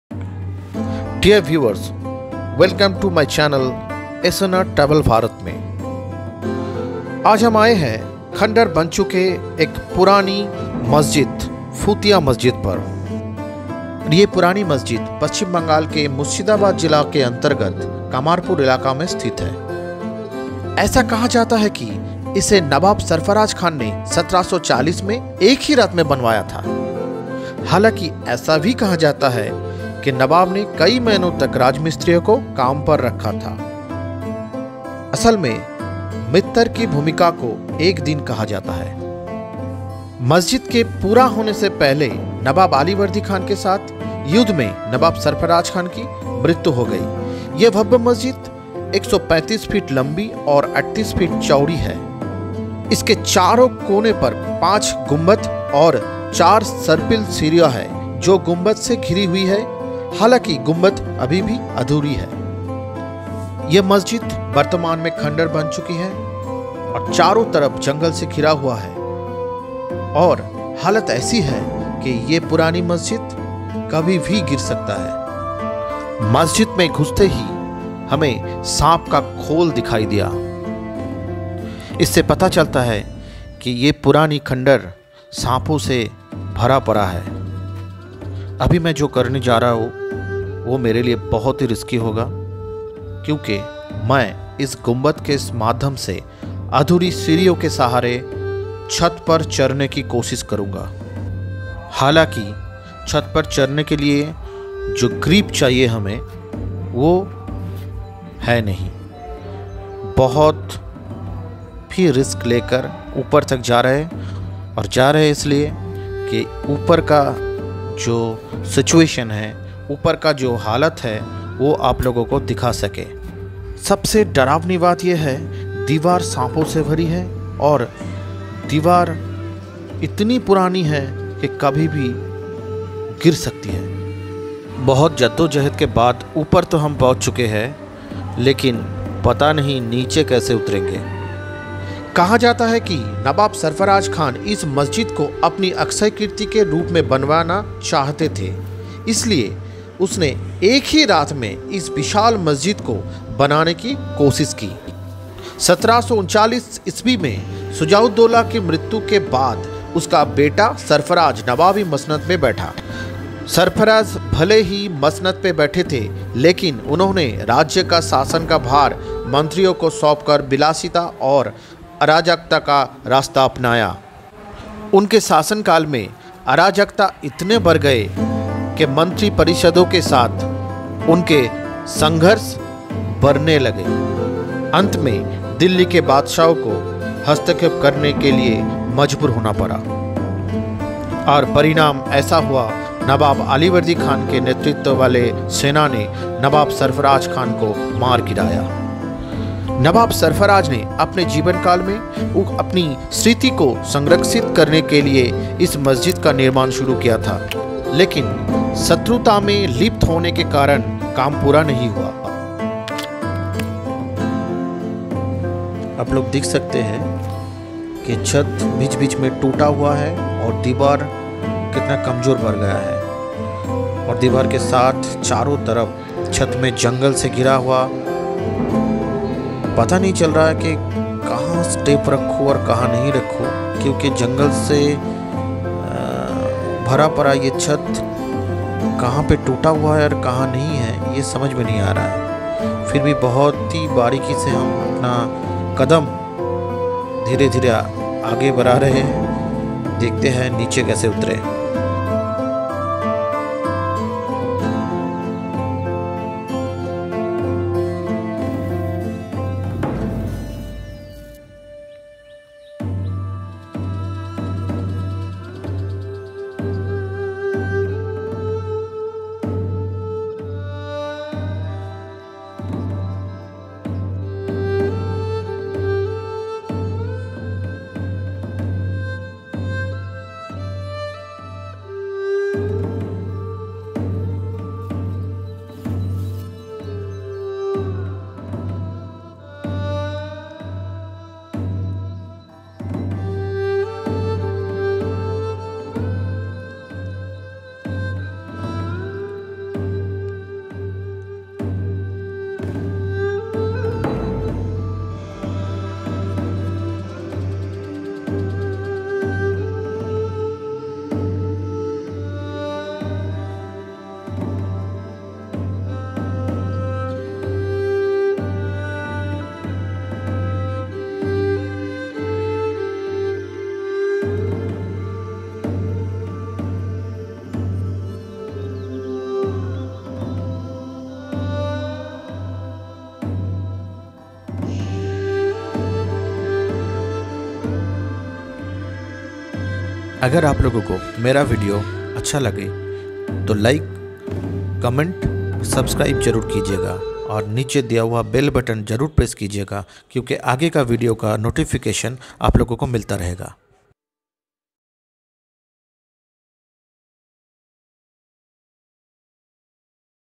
Murshidabad tour/Hazarduari Tour/Fauti Masjid/Lalbab tour/Murshidabad historical place
Fauti Mosque (also Phuti Mosque) is a mosque at Kumarpur in the Murshidabad-Jiaganj CD block in the Lalbag subdivision of Murshidabad district in West Bengal, India. It was built by Nawab Sarfaraz Khan in 1740 AD. The old Fauti Masjid is one of the largest mosques in the town of Kumarpur and Murshidabad district.